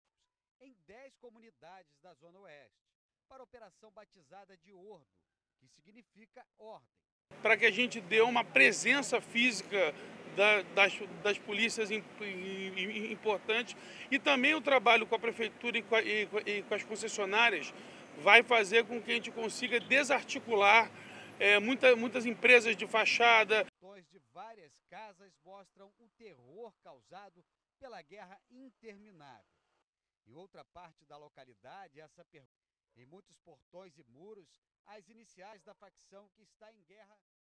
Brazil police launch a mega operation in Rio de Janeiro against organized crime
Brazil police launch mega-operation in favelas in Rio de Janeiro in battle against organized crime.